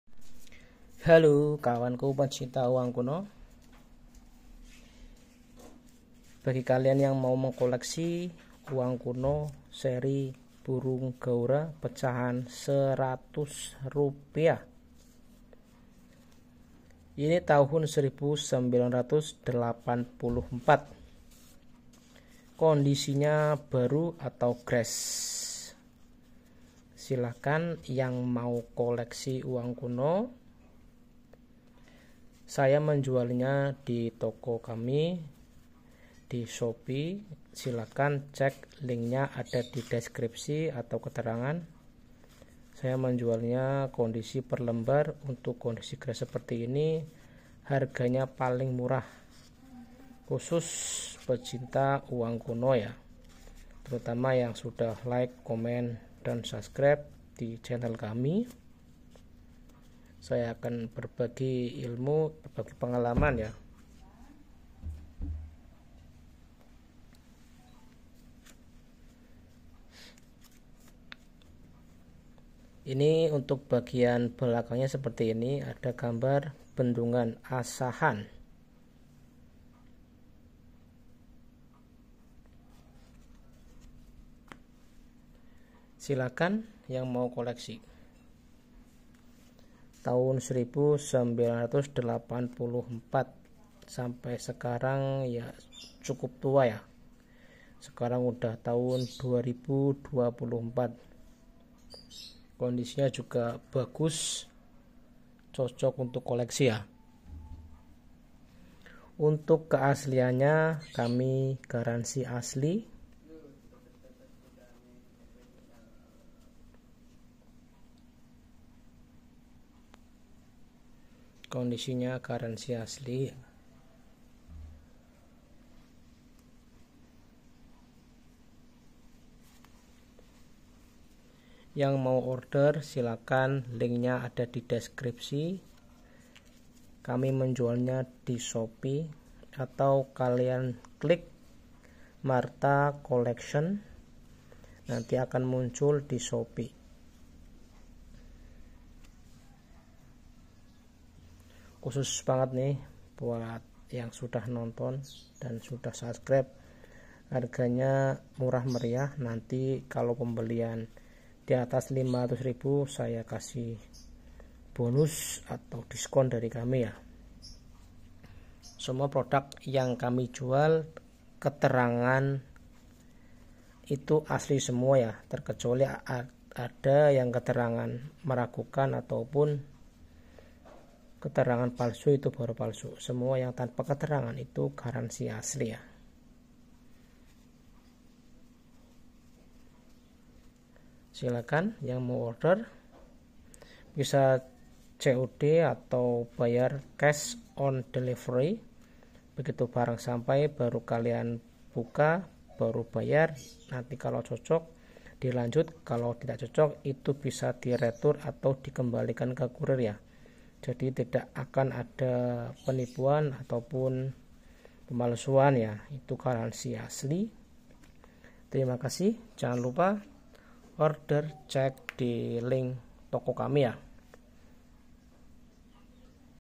Mengenal Uang Kuno 100 rupiah Gaura victoria tahun 1985 Asli
uang Kuno indonesia pecahan 100 rupiah gaura raflesia tahun 1984
bagi kalian yang minat silahkan kunjungi toko kami di shopee silahkan
terimakasih